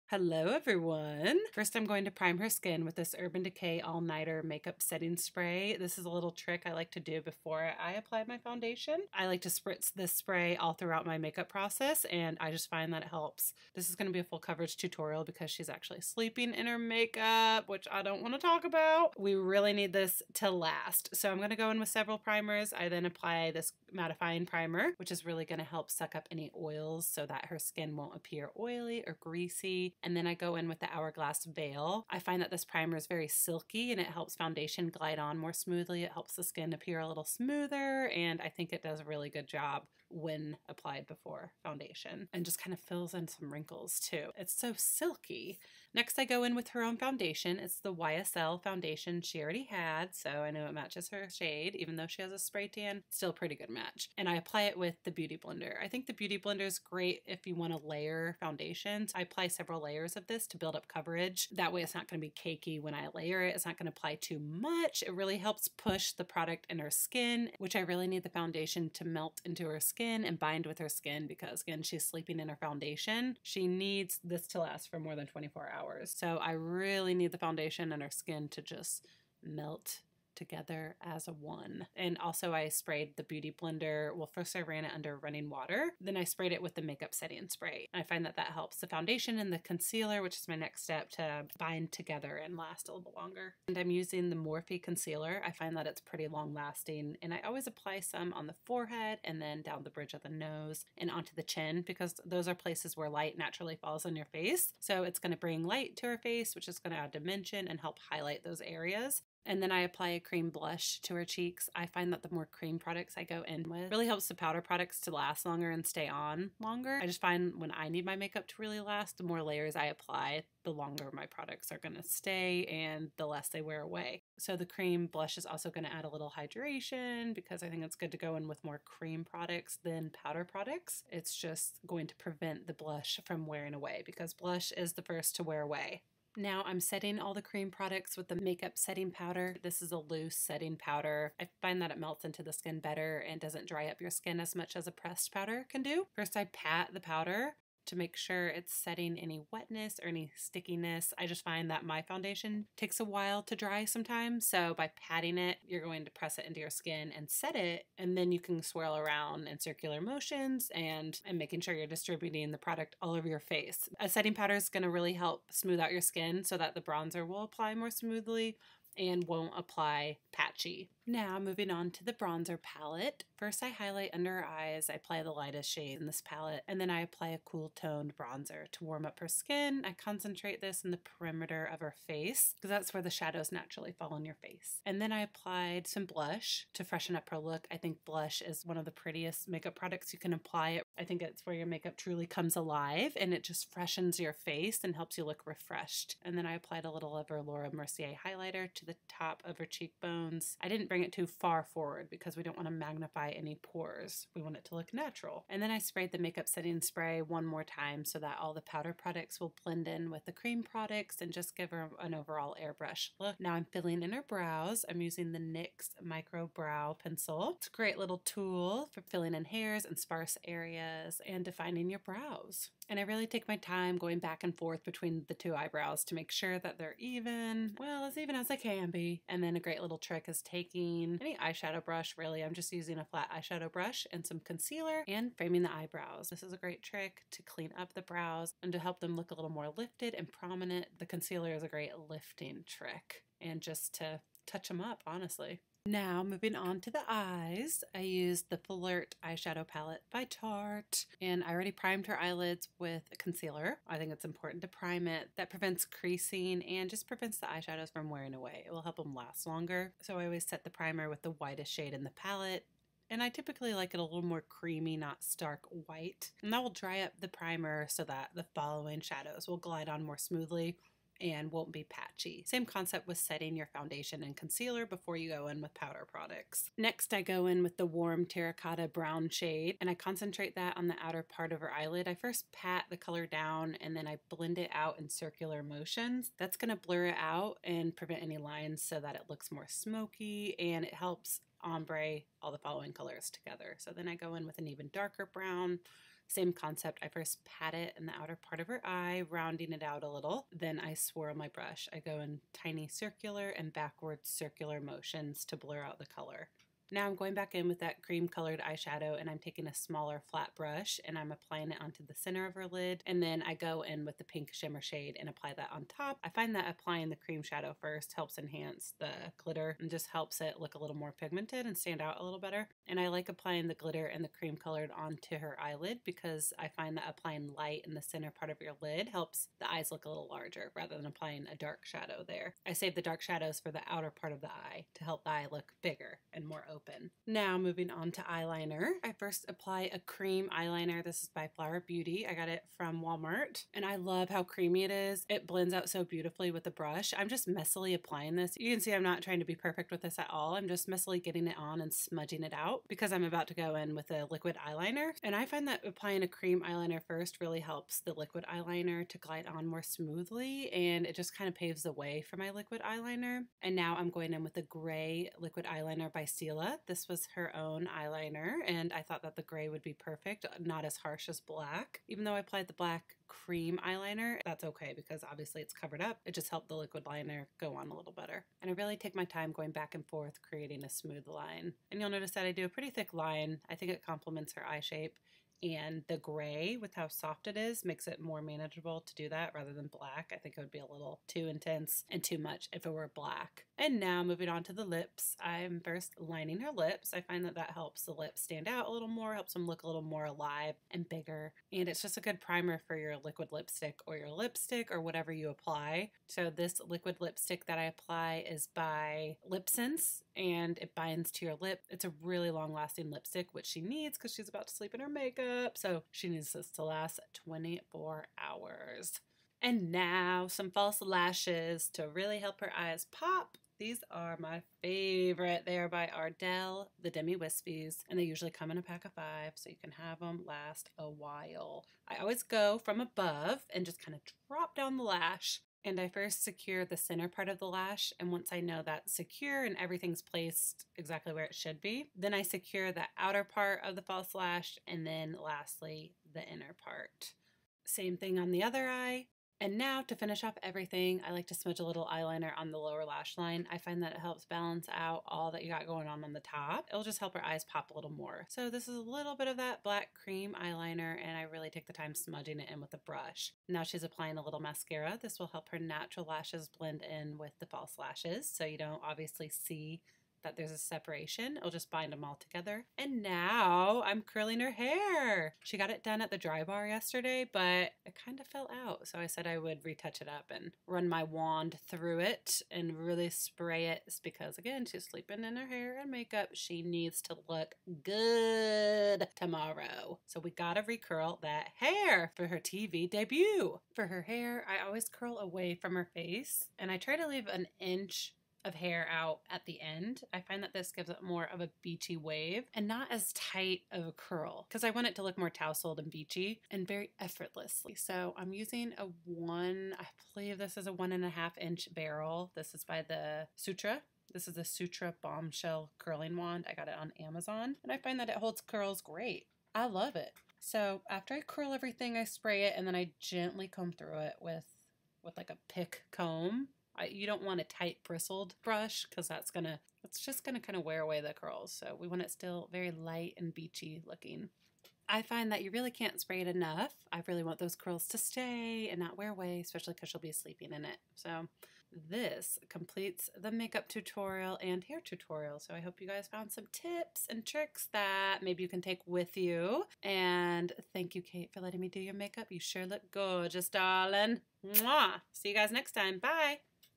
Long Lasting Makeup Tutorial! Tips For how you can make your makeup Last for 2 DAYS
Very exciting video today! In today’s video I show you how you can achieve long lasting makeup that can last up to 2 days! Although I do NOT condone sleeping in your makeup, desperate times call for desperate measures.
I had the privilege of dolling up my sister in law today! She is a professional organizer here in Indianapolis, the EveryHomeOrganizer, and she was a featured speaker in our Indianapolis home show. She was asked to speak 2 days in a row on what inspired her to start her own business, and also was interviewed at our local news channel!
She had busy back to back days so I did her makeup on Thursday for her stage debut. She had to be at the news station early Friday morning and then the home show after that to speak.
I share with you tips on how to make your makeup last and look professional.

Makeup Setting Spray Primer

Cane and Austin Primer

Hourglass Primer

Flat Foundation Brush

YSL Foundation

ELF Cream Rose Royalty Blush

Loose Setting Powder

Tarte Eyeshadow Palette

Flower Beauty Eyeliner

Stila Stay all Day Eyeliner

Nyx Micro Eye Pencil

Ardell False Lashes

Awesome Curling Wand I used

LipSense First Love Liquid Lipstick

Remember, you are beautiful, you are strong, and you can do anything you set your mind to. Tell yourself you’re beautiful. Go on, tell yourself already!

Xx Marguerite